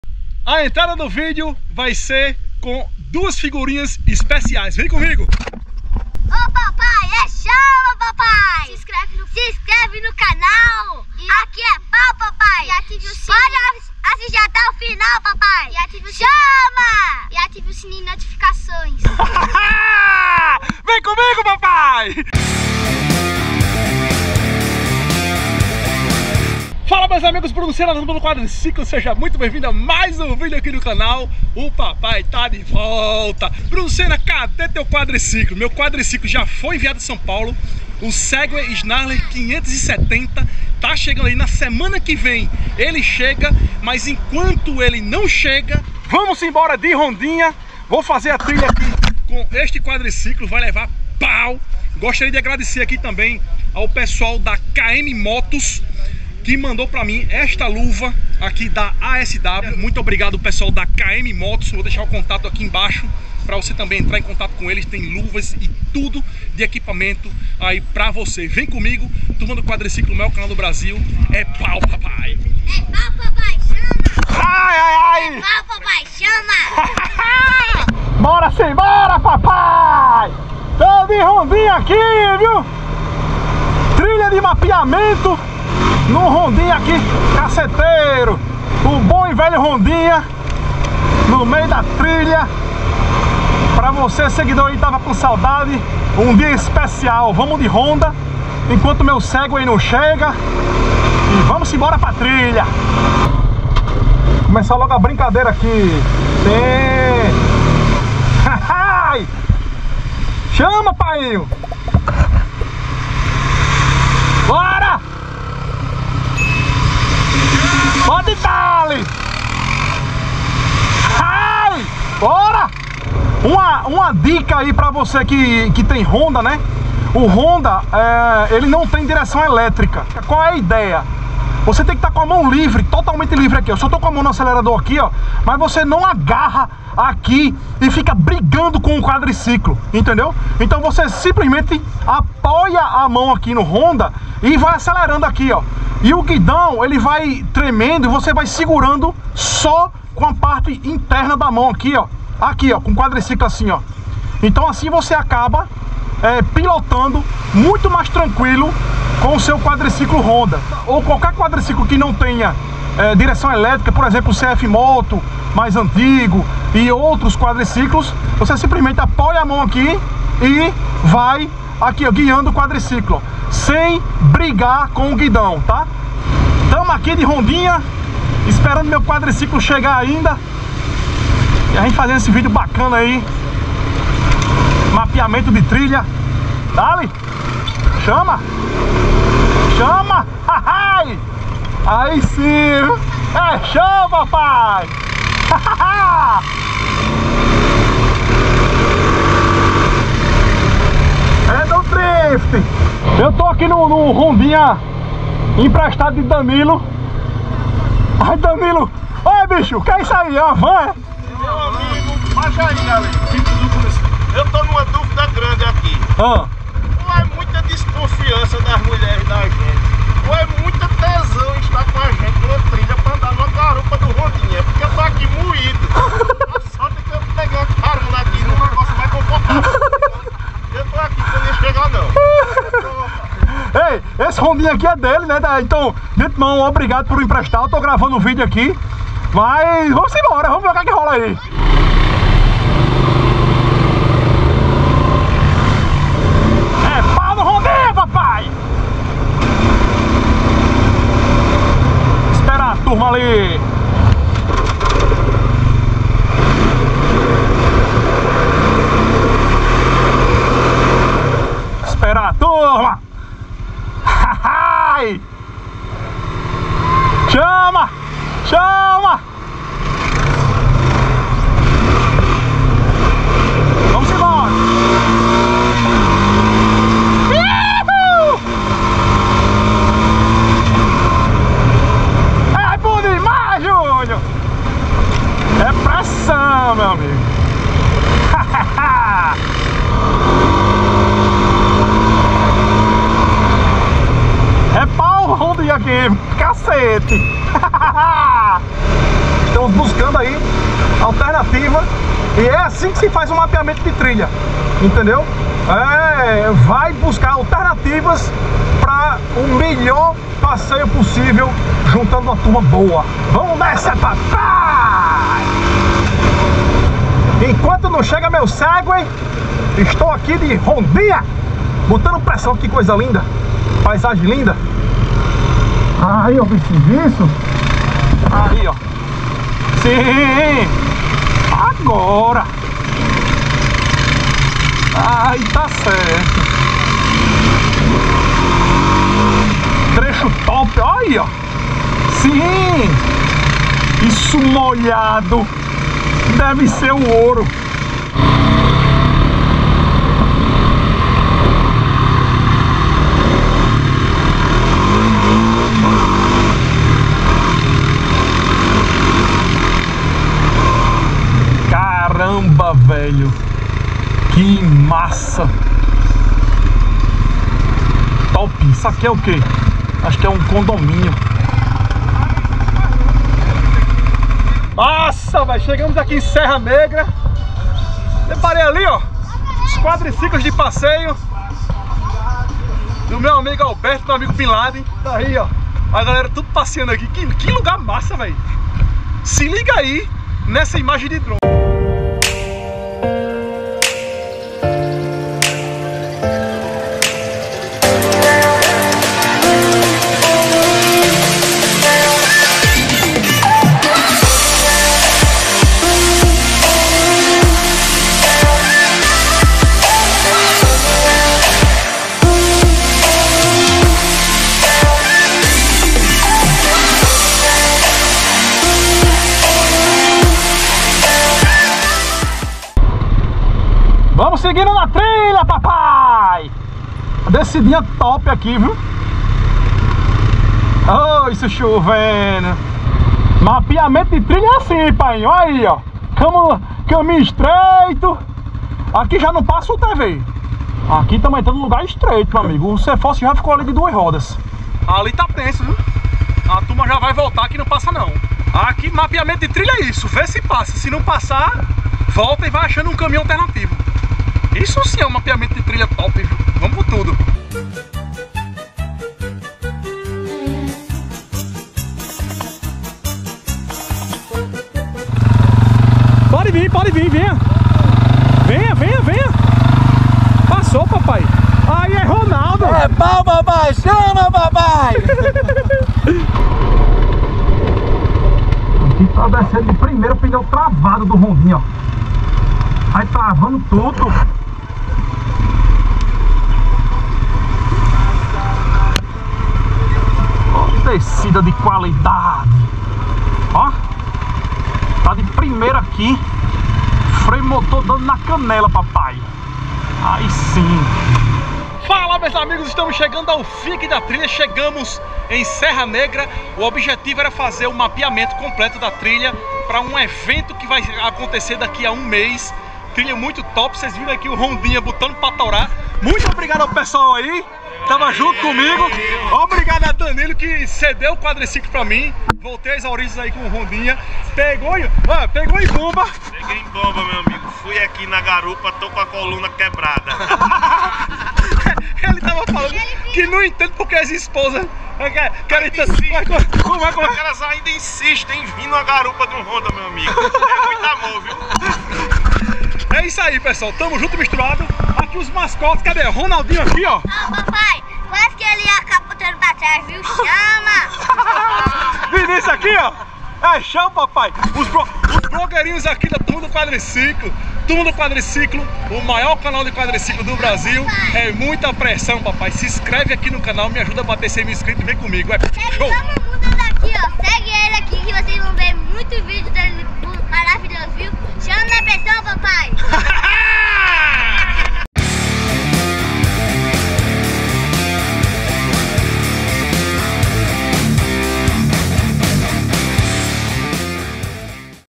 Fiz a trilha completa no Honda e arregacei no zerinho
PARCEIROS da TURMA DO QUADRICICLO

Fabricante de camisas de algodão da Turma do Quadriciclo